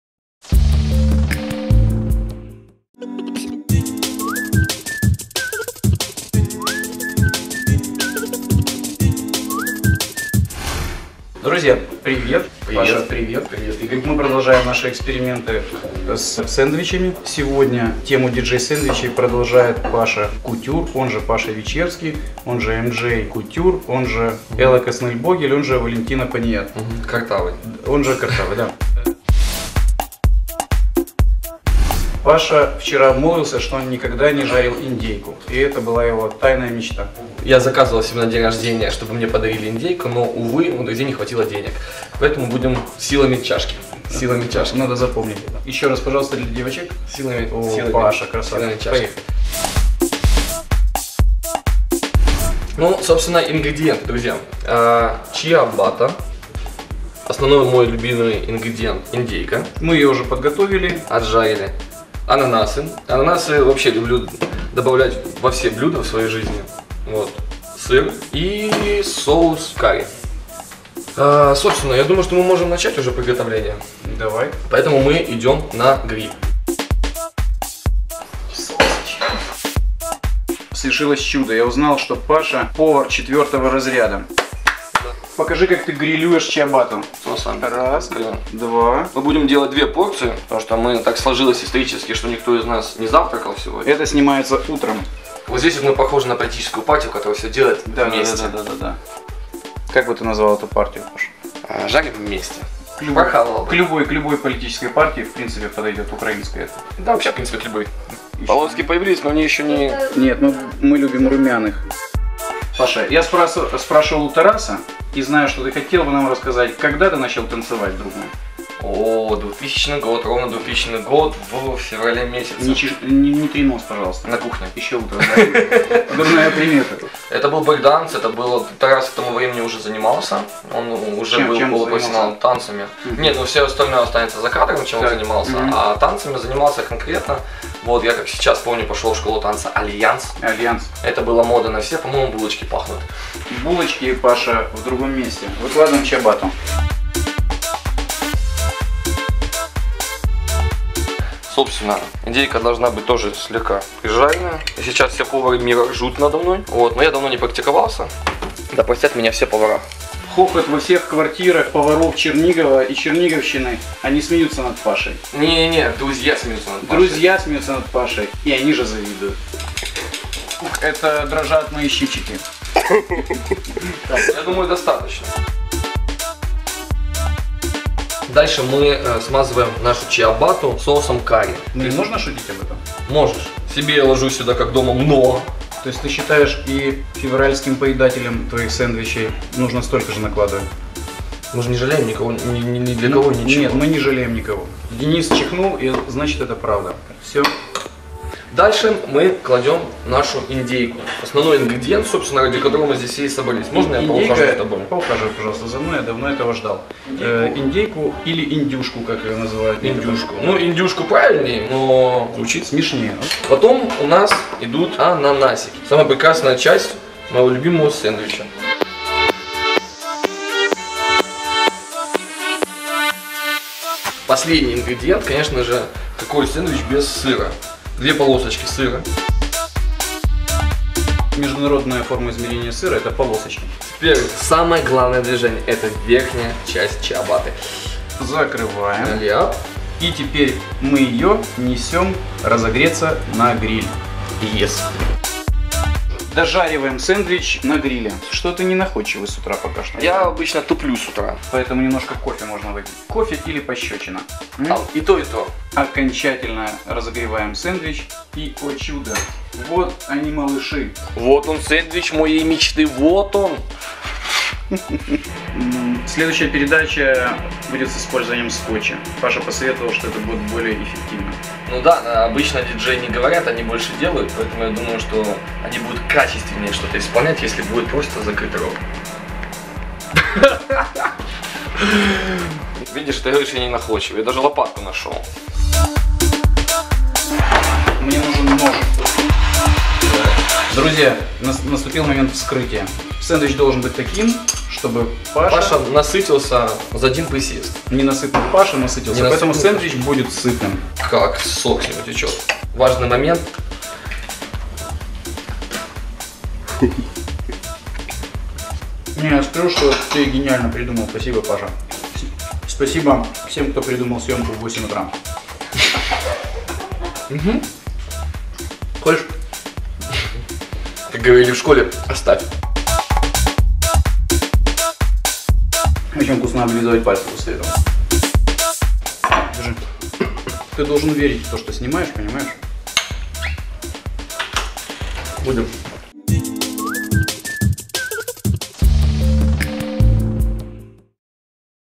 Dj`s sandwich by Mj Couture
Второй выпуск Dj`s sandwich, в гостях Паша "Кутюр". Он готовит авторский сандвич Haute couture с индейкой и ананасом. Себе в удовольствие и гурманам на радость.